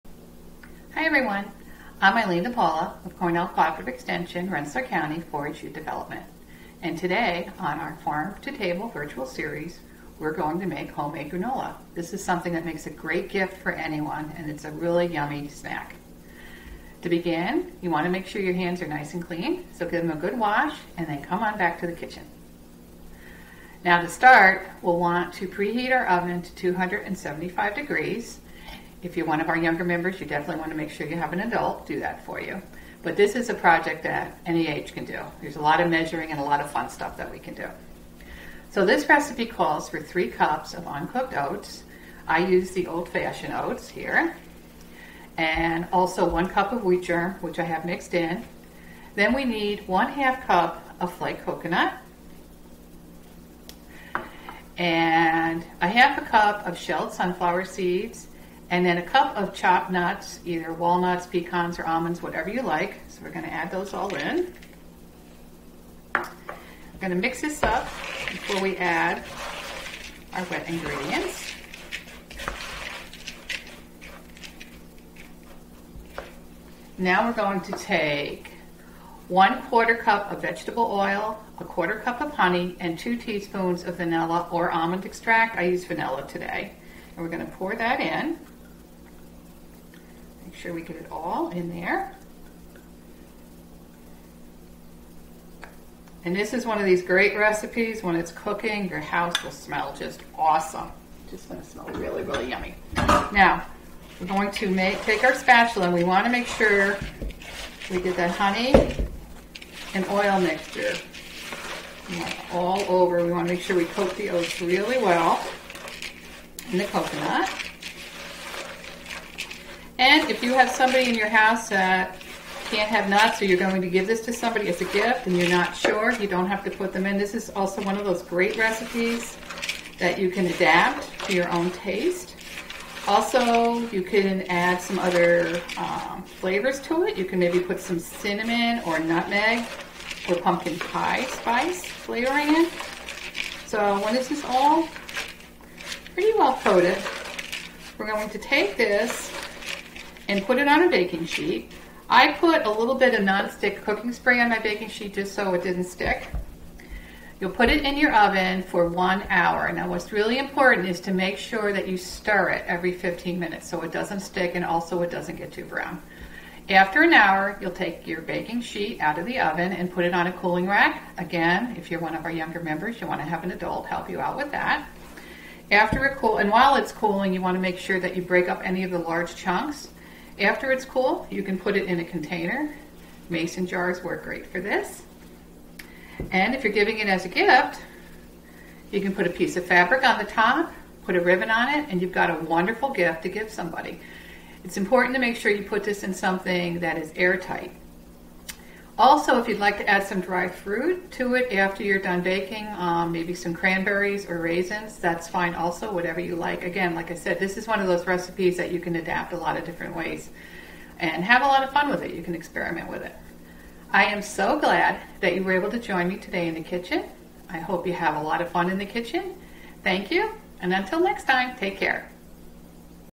Farm to Table Virtual Series: Homemade Granola
Here's a great idea for a delicious holiday treat to make for someone special! Join Eileen to learn how to make your own Homemade Granola!